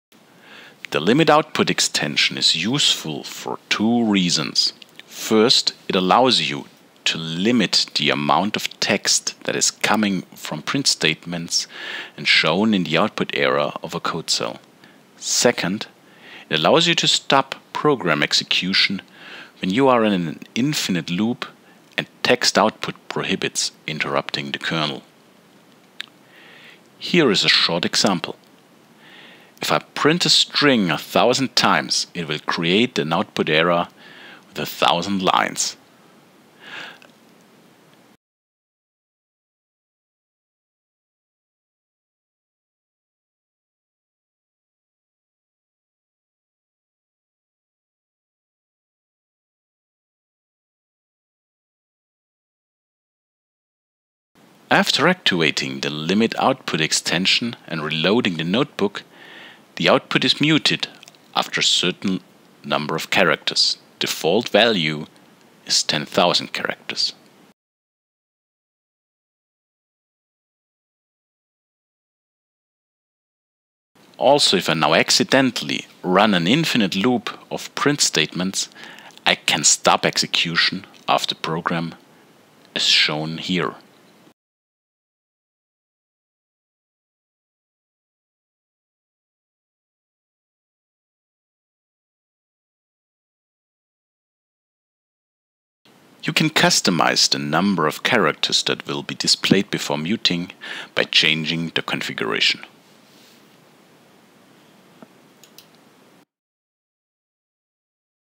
Notebook Extension "limit_output"
Short demo of the IPython/Jupyter notebook extension to limit the number of output characters